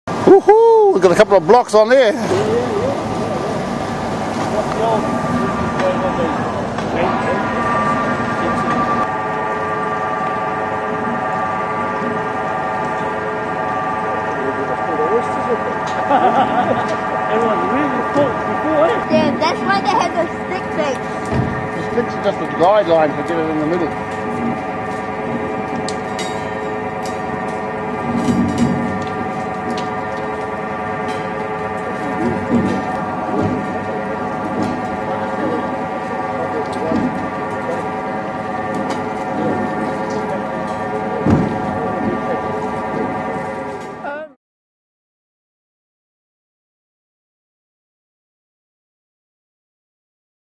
Kohu Ra Tuarua, ferry is coming out of water!! Hokianga, Kohukohu, New Zealand
The video is taken on 9/7/2013 at Kohukohu, Hokianga, New Zealand